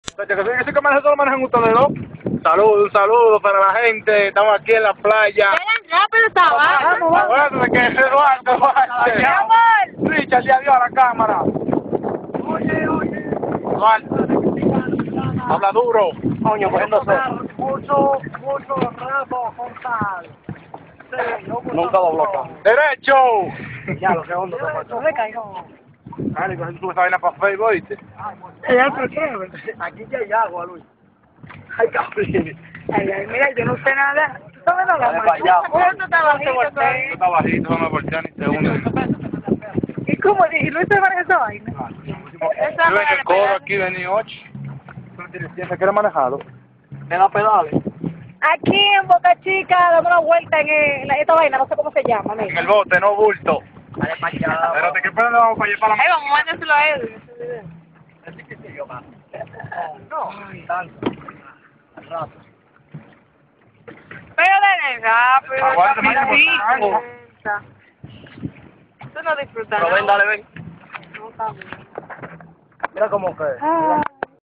SEXIE GIRL IN BOAT BOCA CHICA REPUBLIQUE DOMINICAINE
une journee en pedaleau sur la plage de boca chica